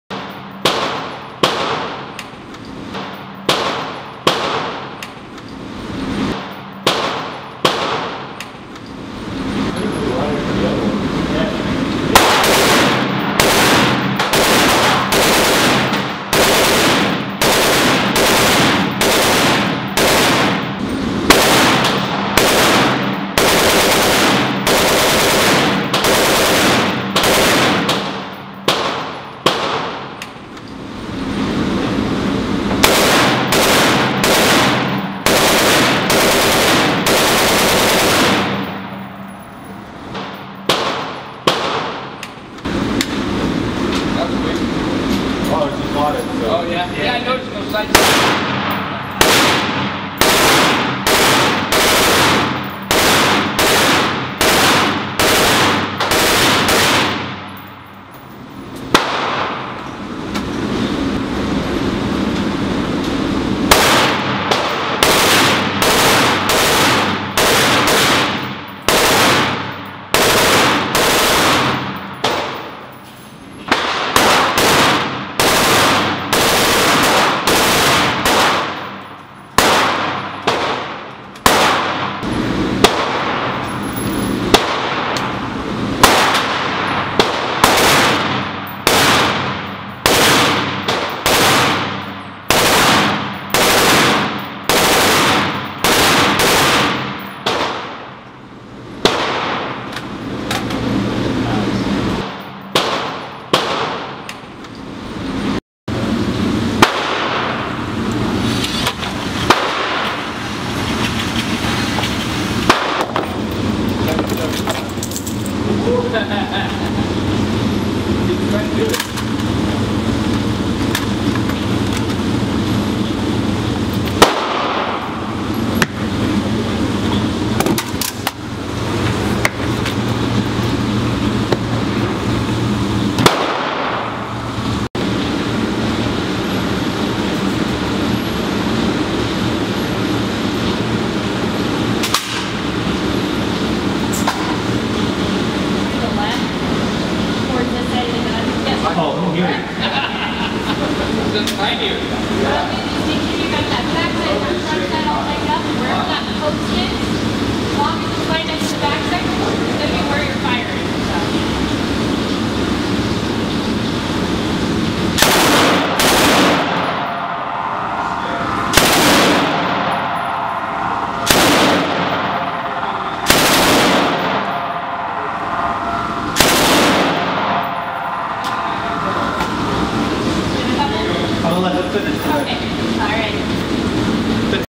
Scottsdale Gun Club:Machine Gun Shooting
Went to Scottsdale Gun Club this past October, 2014 and rented some guns. Love this place and had a great time. Rented the HK G36, Steyr Aug and the S.A.W.